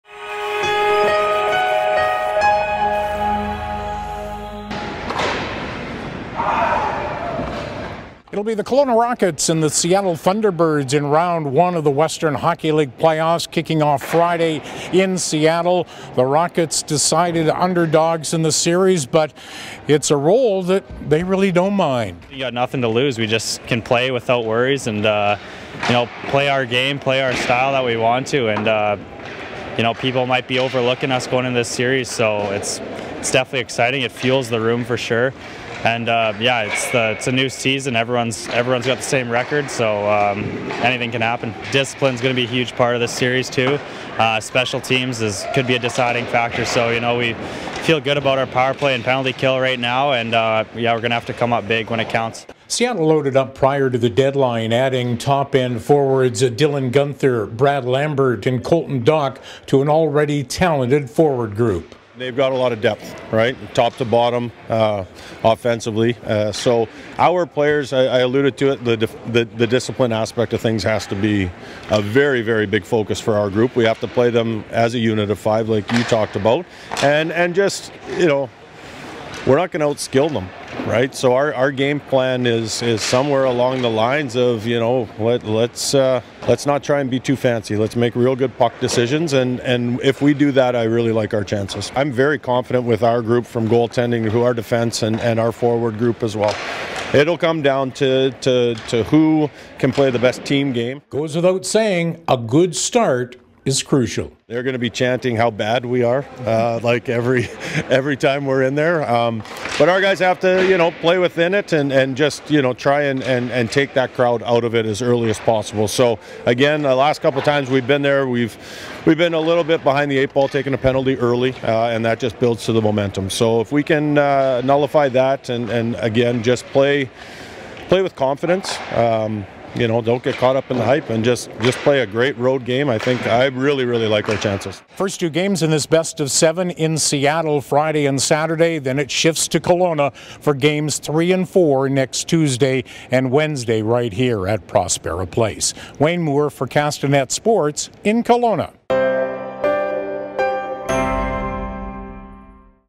Kelowna Rockets prepare for 2023 playoffs
The Kelowna Rockets hit the ice Tuesday morning for practice ahead of their road trip to Seattle, where they'll be taking on the Thunderbirds in round one of the WHL playoffs.